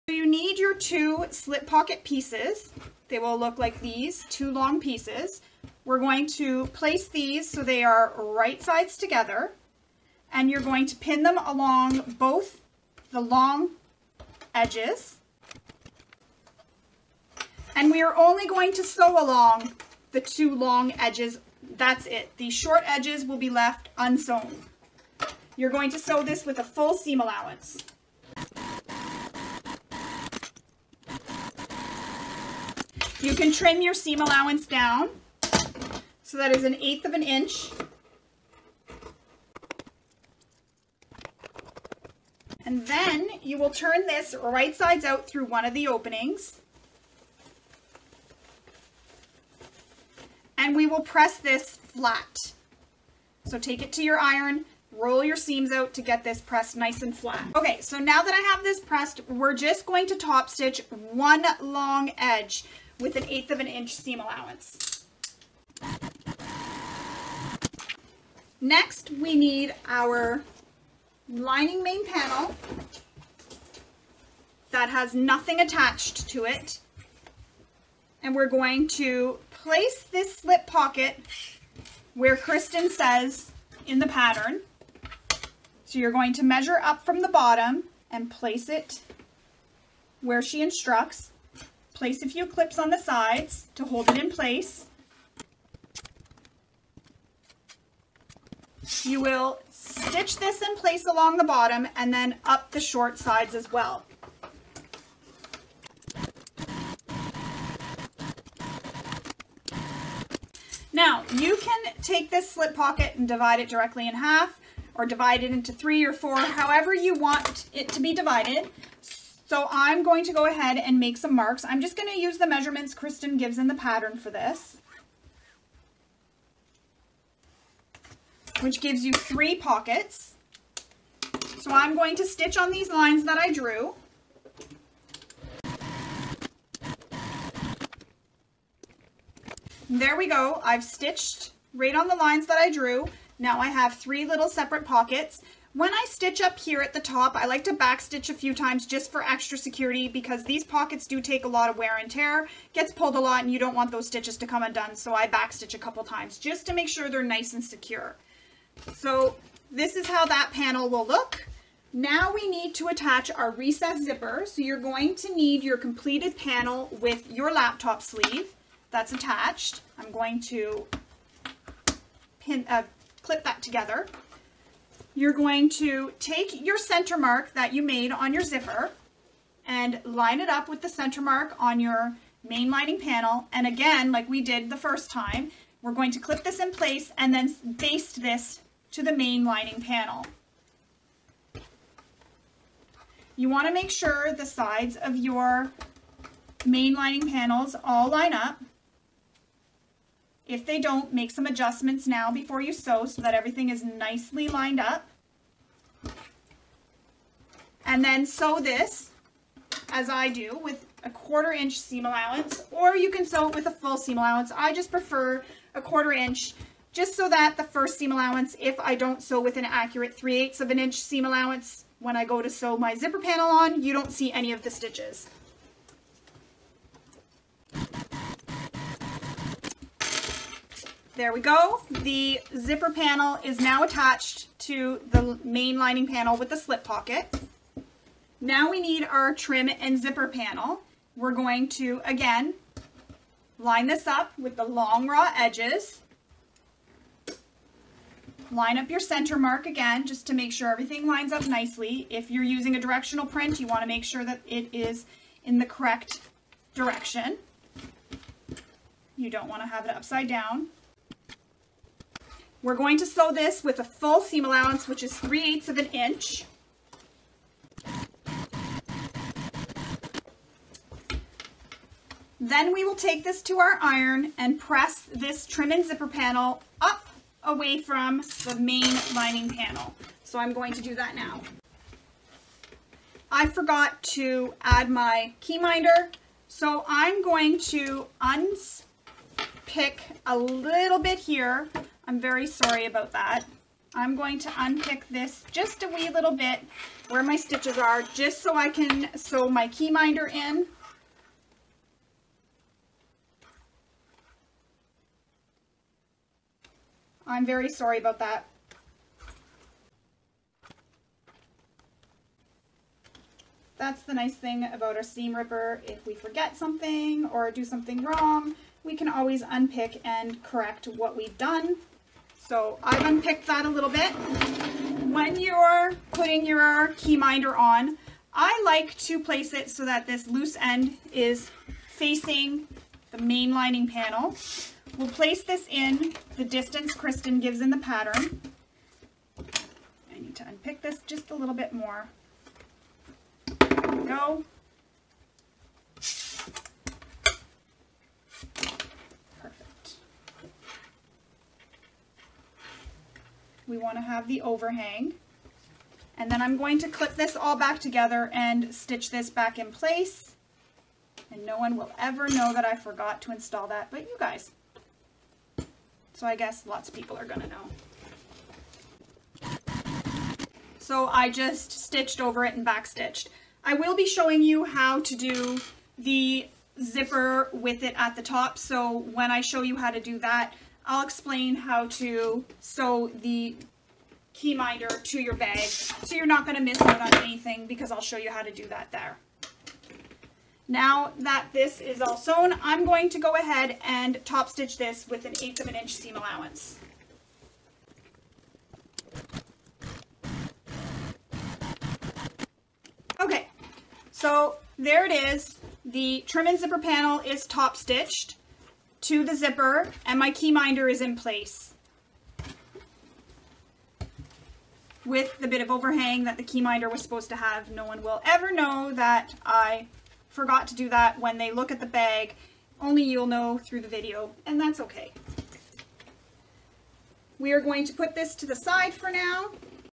Compass Business Bag - Slip Pocket
In this video, I show you how to construct and attach the Slip Pocket and finish attaching the Recessed Zipper Panel.

NOTE  - I had forgotten to add my Key Minder before I attached my Recessed Zipper Panel and TZP. But  I do show how you can add it after you've sewn the TZP to the MLP with the Recessed Zipper attached, by unpicking a few stitches with your seam ripper and then adding your Key Minder.
I have a separate video, showing how to add the Key Minder, the way Kristin instructs in the pattern, so that you can see how it is done.

The machine I sewed these bags on is a Juki TL-2200 QVP Mini and I used 90/14 needles. For threads I used Gutterman All Purpose on the Science themed bag and on the Tula Pink bag I used Tex 45 Fairy Floss from Wizardry Stitchery. You'll want to use the needle and threads that work best for your machine.

Where to find the pattern and supplies I use...
Pattern by KMGHandmade

Deep Space Soft Vinyl and Black Moonglow Vinyl From Sew HungryHippie